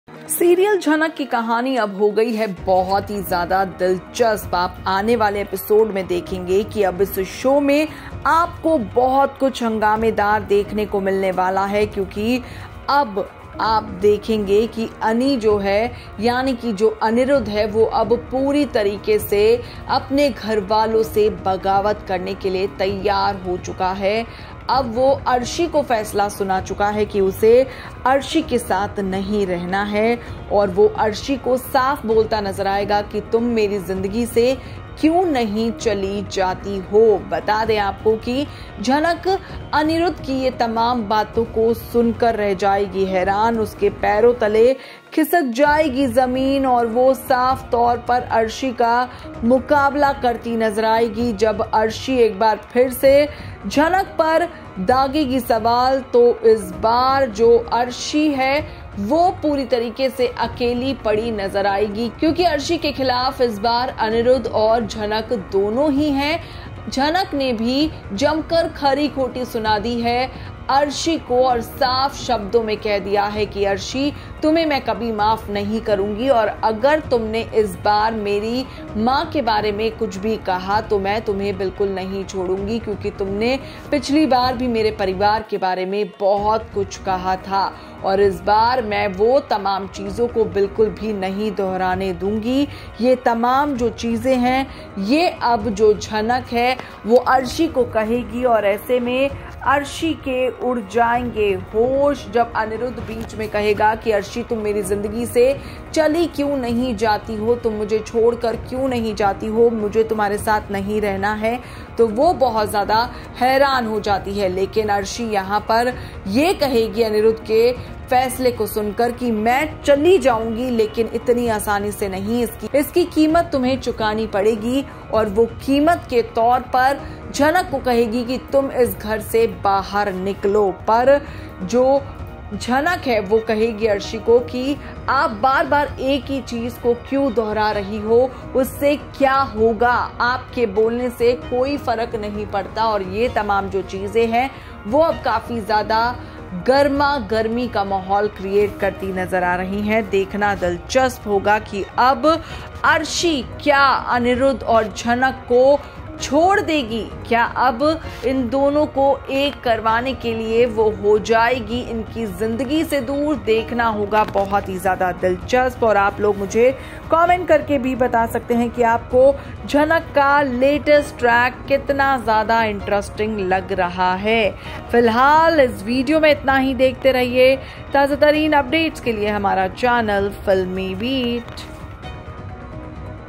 Jhanak Spoiler Update:Jhanak के लिए Aniruddha ने Arshi से खत्म किया रिश्ता ! | FilmiBeat
Jhanak Spoiler Update: Aniruddha ready to leave Arshi for Jhanak .  Arshi gets angry. Watch the video to know more about this !